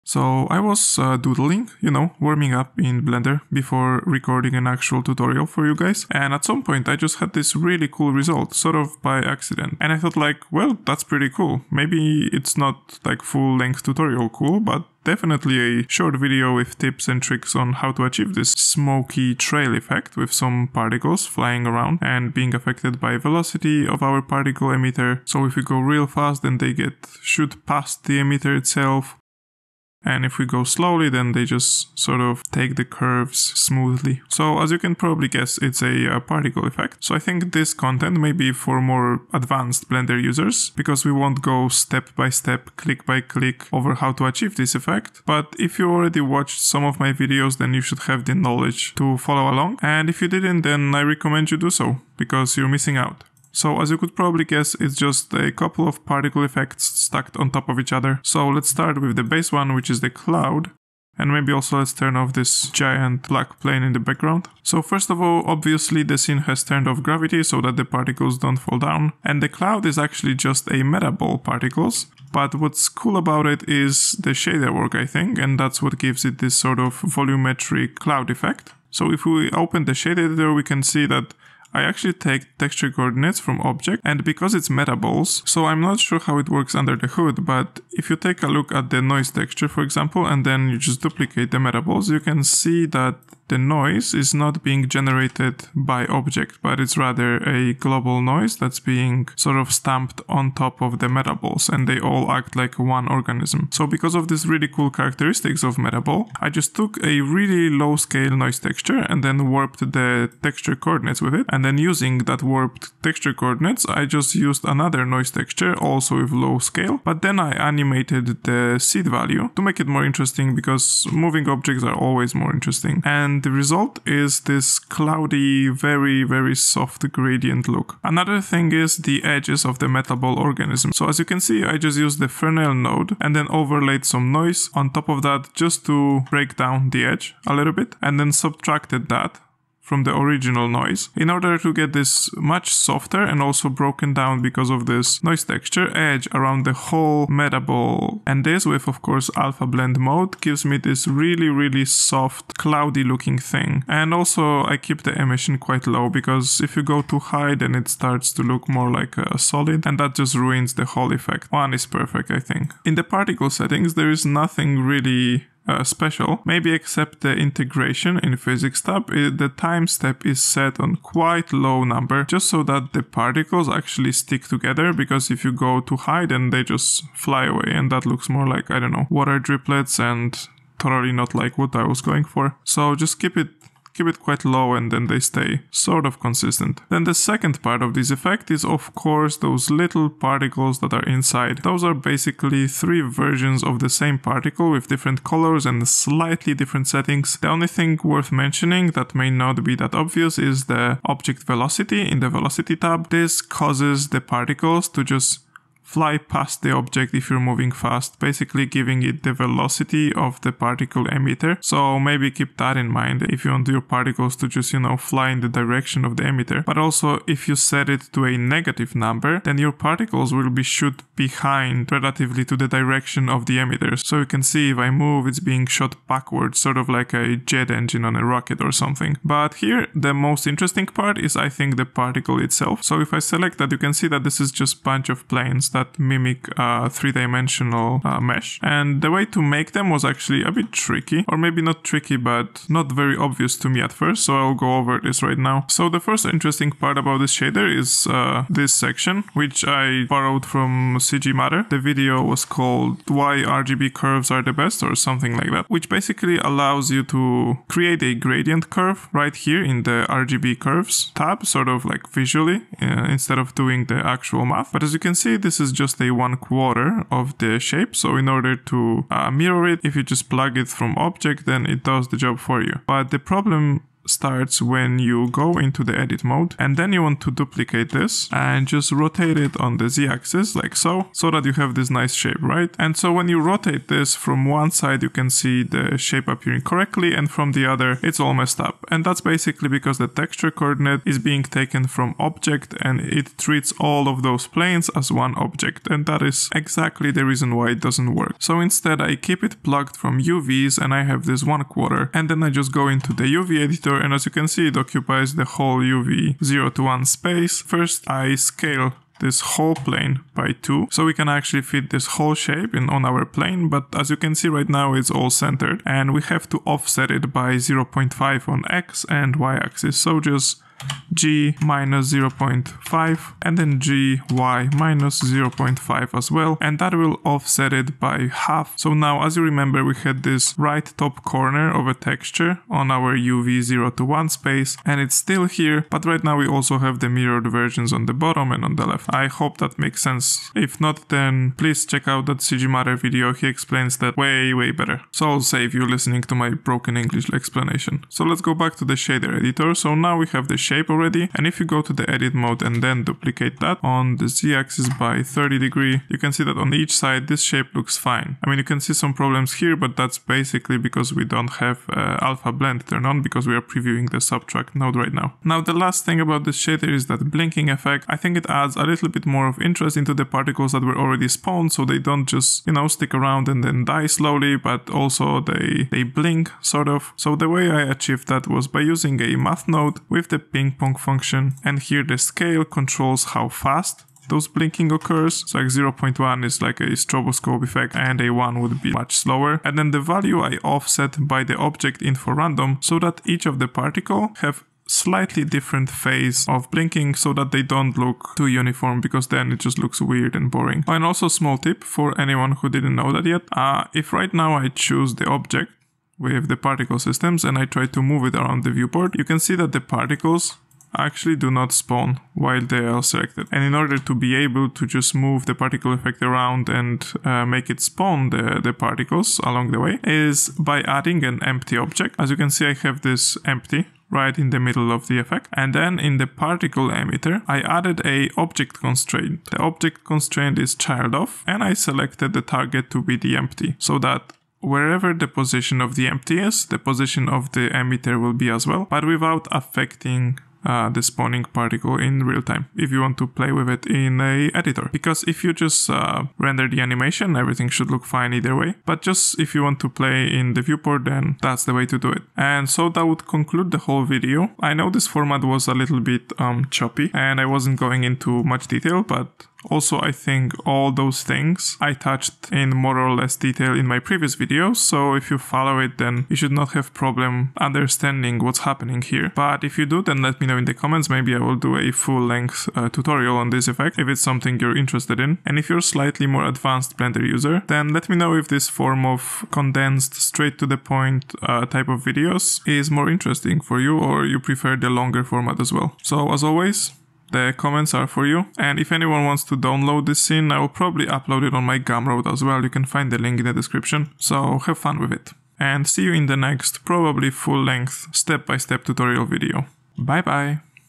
Sailor Moon inspired Celestial Trails Particle Effect in Blender - TIPS
just a quick hot take of what I was working on, with some tips.

Let me know how do you like this format, or would you prefer the long step-by-step tutorials more.

If you manage to make something out of this, share it with us !

If you don't feel like following the tutorial, it's ok. Life is Pay to Win kinda game so you can grab the project file here:

Learn how to Animate like a PRO from one of the Best in the industry: